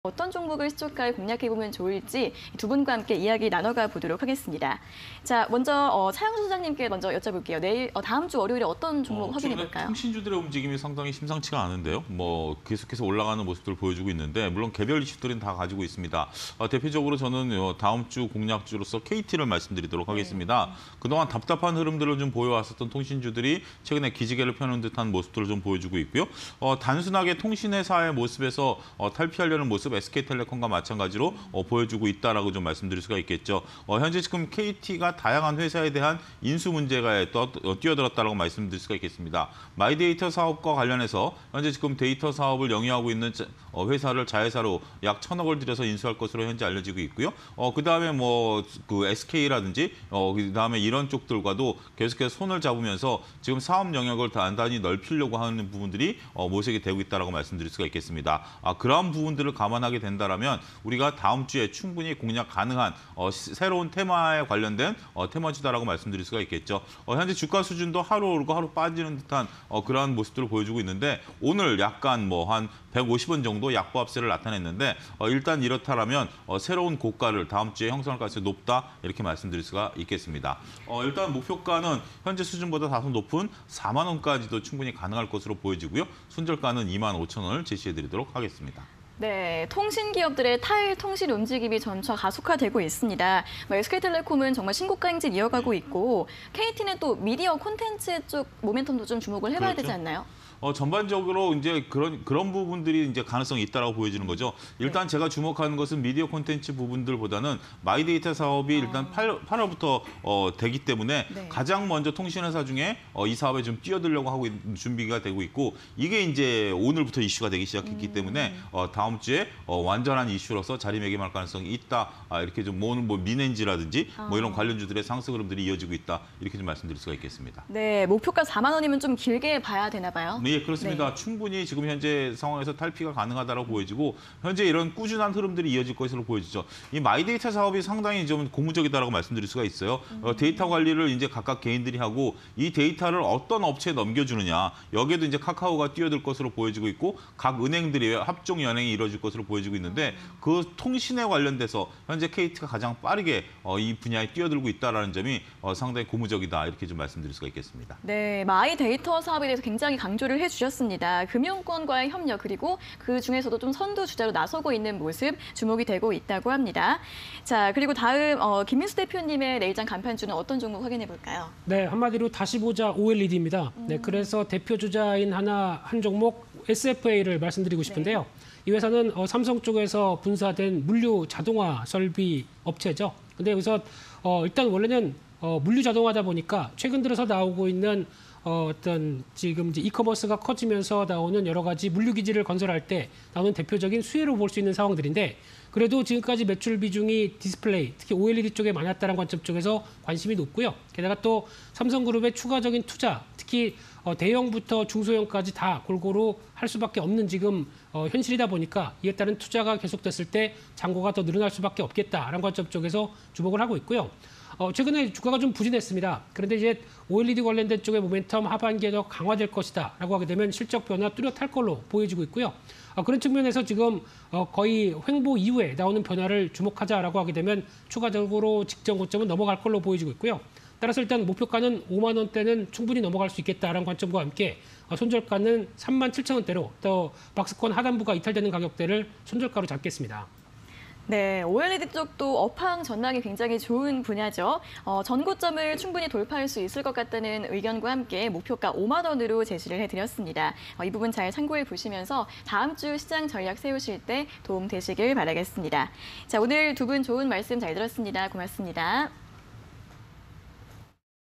KT, 에스에프에이_내일장 간판주 (20210416)
KT(030200)
- 통신기업 탈피 선언… 디지털플랫폼으로 변화
- '뱅크샐러드' 등 다양한 회사 인수합병 기대
- 본인 신용정보관리업(마이데이터) 사업 진출 기대

에스에프에이(056190)
- SK이노베이션 공급 2차전지
- 3D ICT 비파괴 검사기 부각
- 올해 실적 2019년 수준 회복 기대